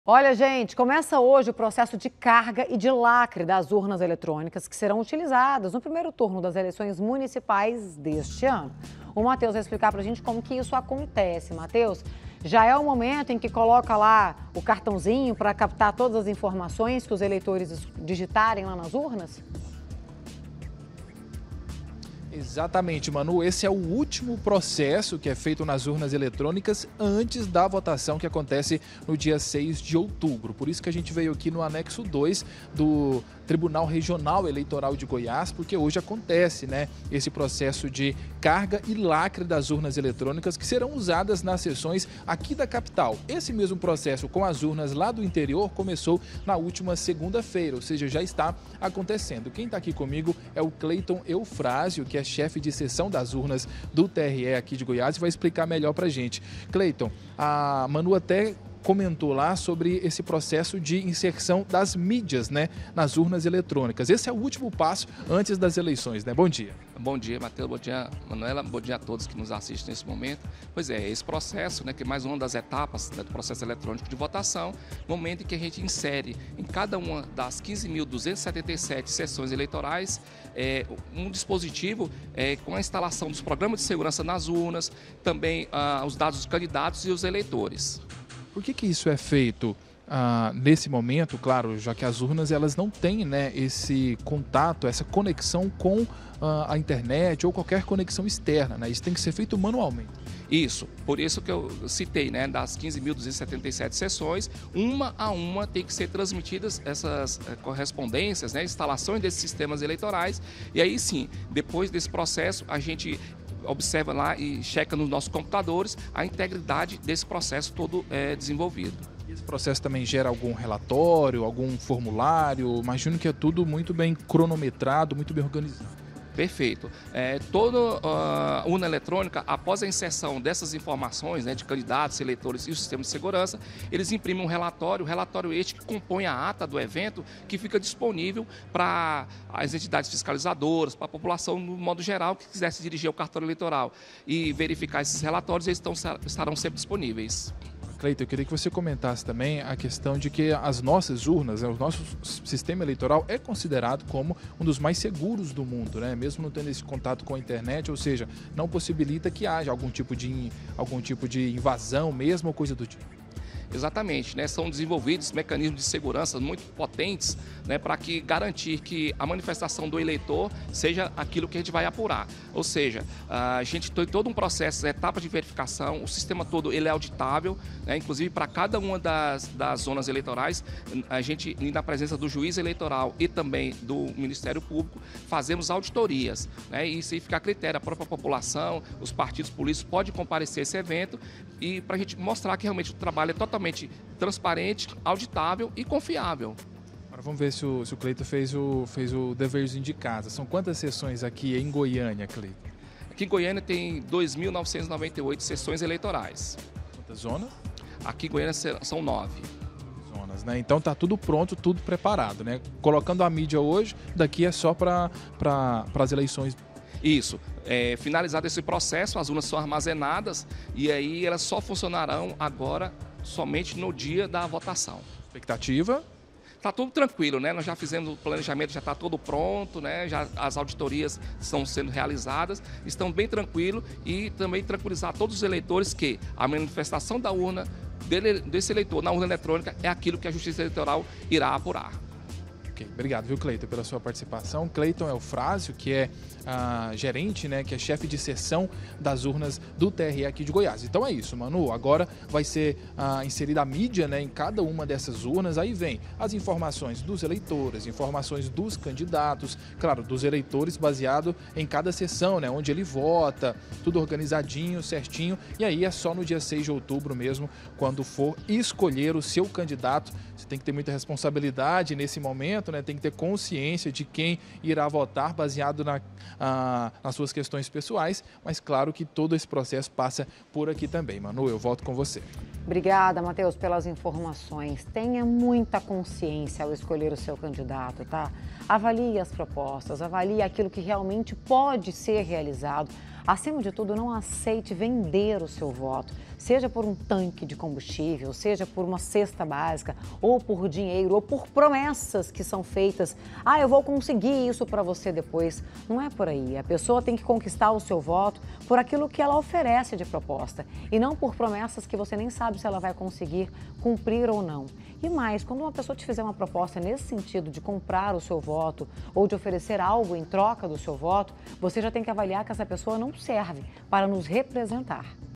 ELEIÇÕES 2024: URNAS SERÃO LACRADAS HOJE
COMEÇA HOJE O PROCESSO DE CARGA E LACRE DAS URNAS ELETRÔNICAS, QUE SERÃO UTILIZADAS NO PRIMEIRO TURNO DAS ELEIÇÕES MUNICIPAIS 2024.